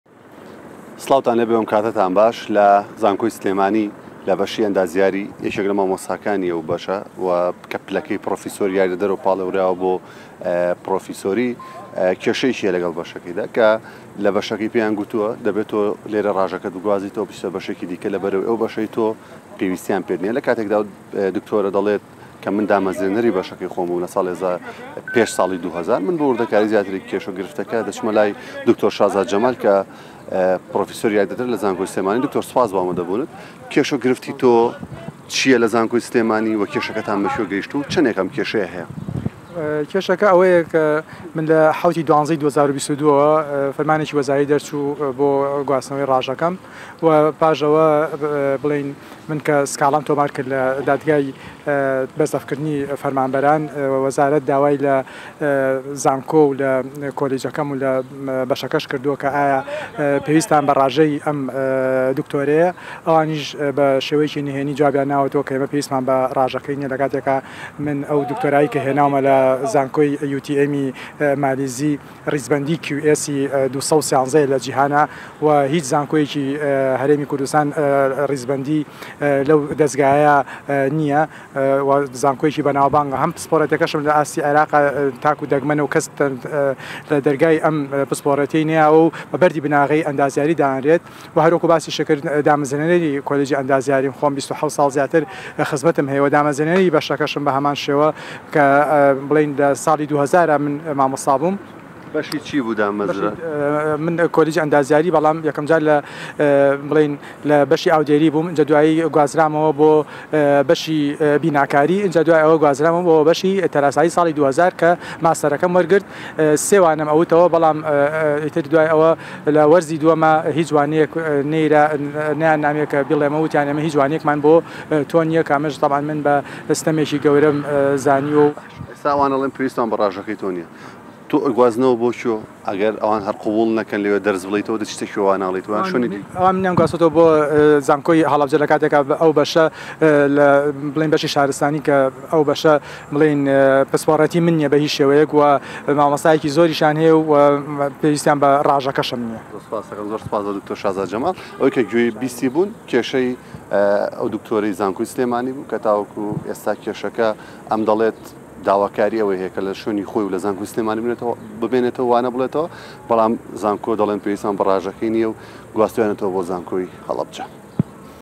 ئەم مامۆستایەی زانکۆی سلێمانی لە بەشی ئەندازیاری :"دامەزرێنەری کۆلێژی ئەندازیاریم و دوورخرامەوە"
ئەم مامۆستایەی زانکۆی سلێمانی لە بەشی ئەندازیاری دەڵێت، "دامەزرێنەری کۆلێژی ئەندازیارییە" و دووردەخرێتەوە: پسپۆڕییەکەم لە عێراقدا تاک و دەگمەنە